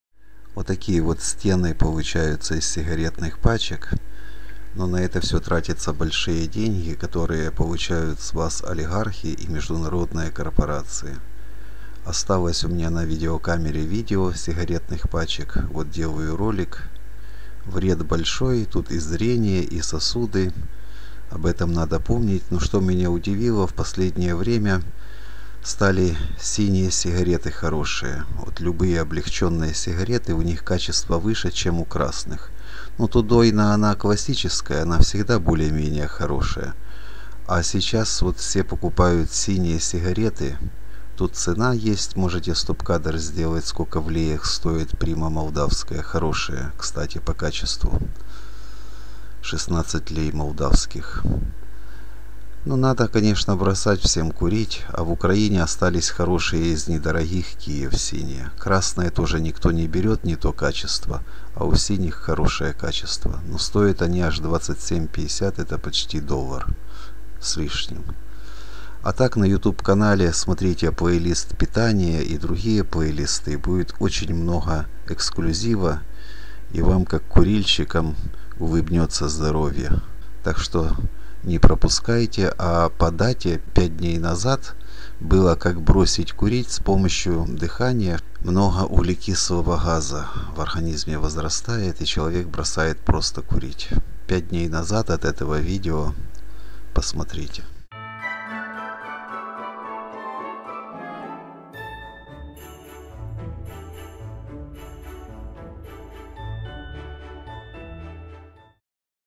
Синие лучше красных сигарет Тенденция однако
На рынках продаются молдавские сигареты и с РФ.
Качество, лучше чем то, что производят в Украине.
Будет на канале: природное питание для продолжительной и качественной жизни и эзотерика (спиритизм, таро).